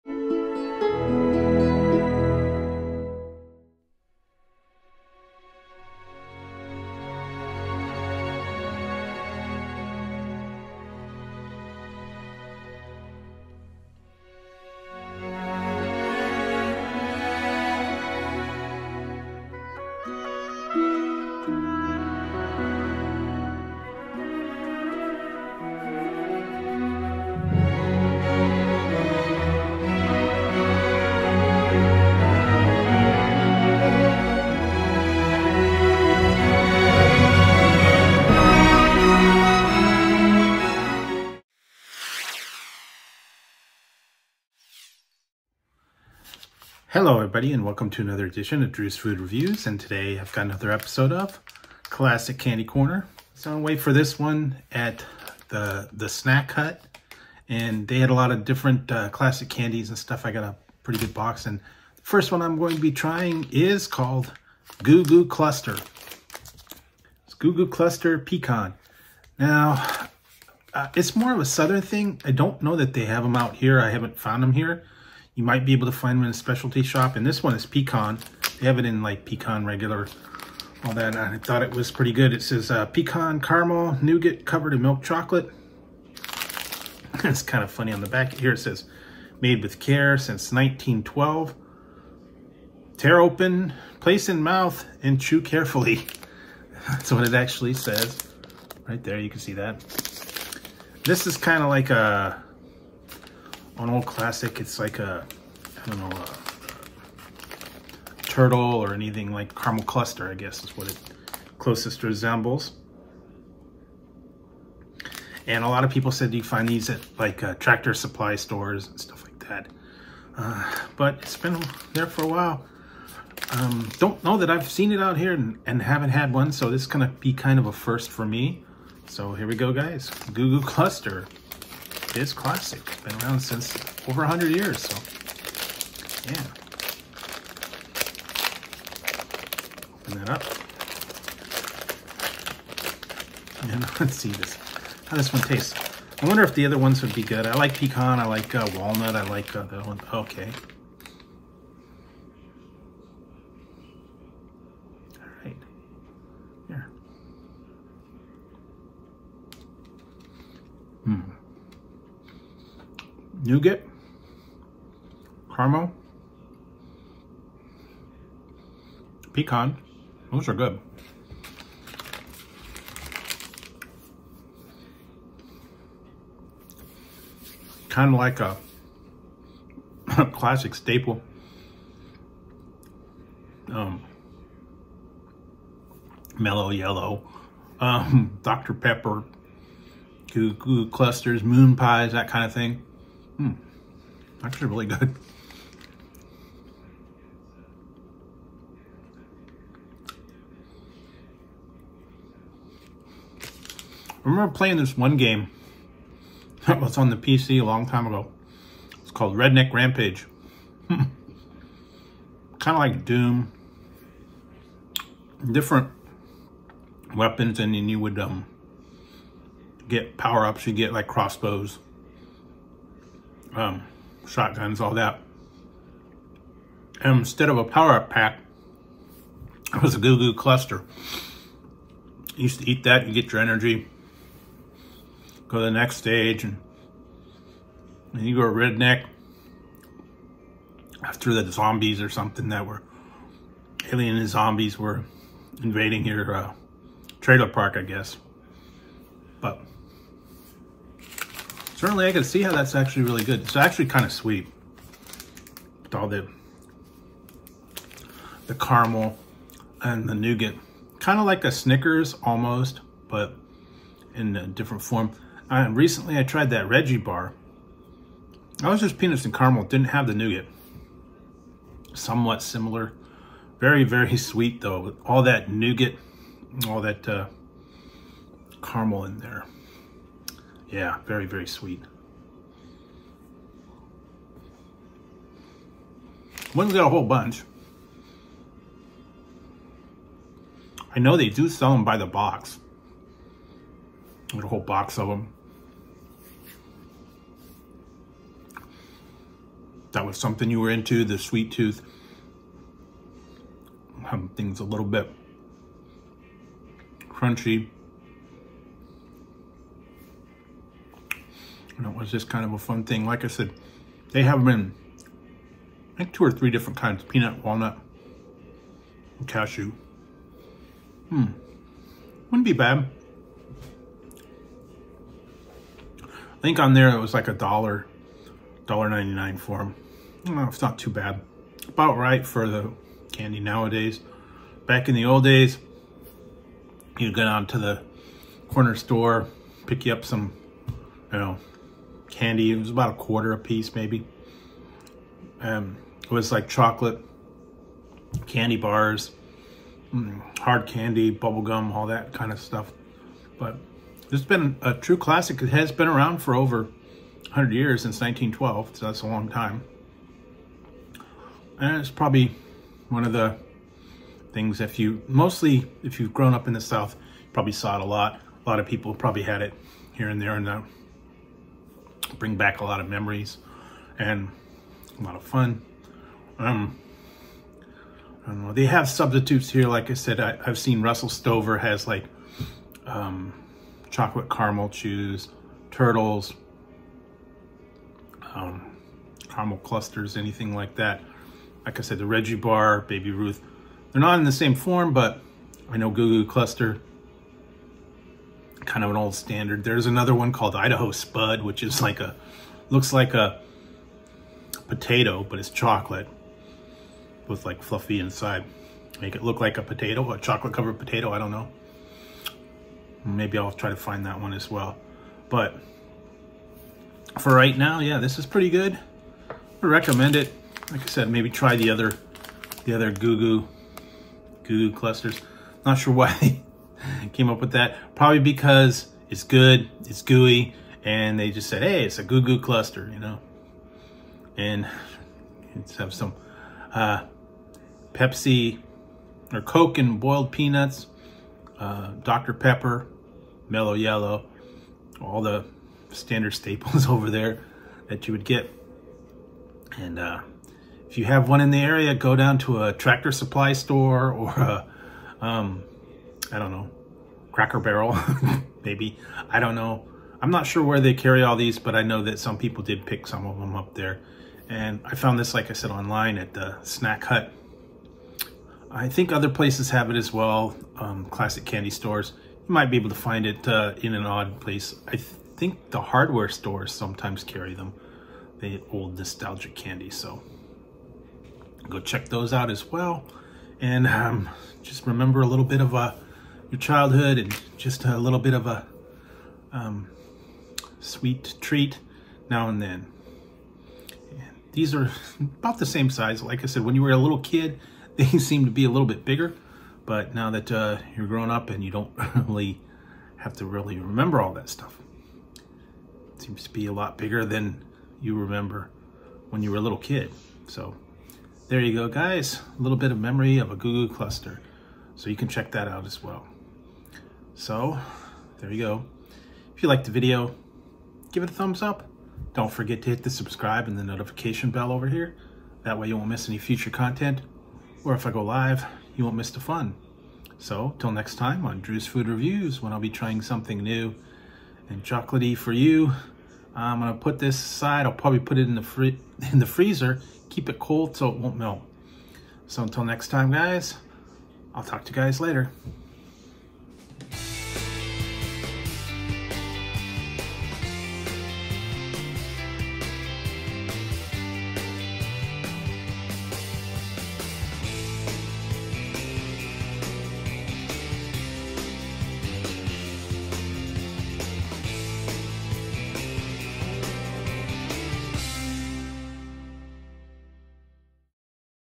Classic Candy Corner : Goo Goo Cluster Pecan
Today on Classic Candy corner we have a 100 year old treat the classic Goo Goo Cluster with pecans . It’s a combo of nougat caramel pecans covered in milk chocolate. Let’s check this one out .